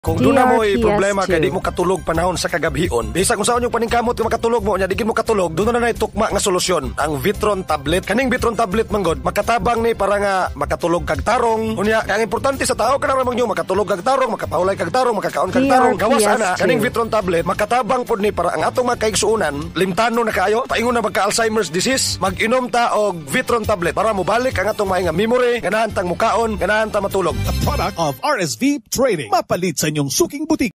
Vitron Food Supplement Radio Ad 2022-2023 31s (in Cebuano/Main Version)
Ito po ang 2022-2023 Main Radio Ad ng Vitron Food Supplement na gawa ng RSV Trading.

DRPS Two 4116: Vitron Food Supplement Radio Ad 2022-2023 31s (in Cebuano/Main Version)